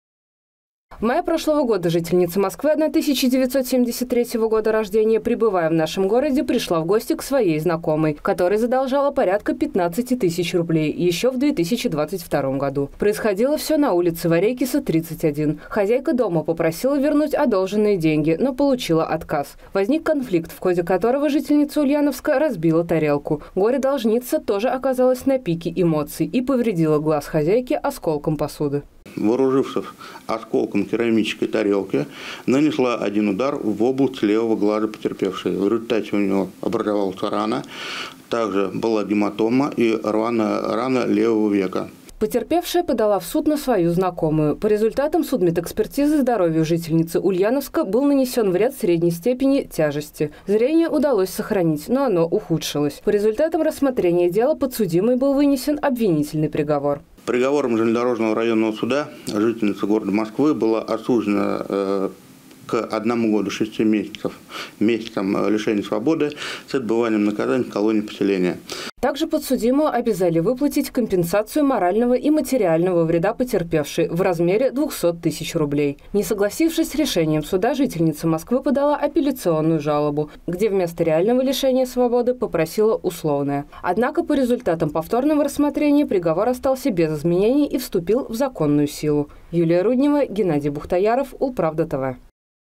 Тарелкой в глаз. Москвичку лишили свободы в Ульяновске
Гостья из столицы, занявшая деньги у жительницы Ульяновска, доставила последней множество проблем, и не только финансовых. Горе-должница и деньги отказалась вернуть, и побила приятельницу. За что и лишилась свободы.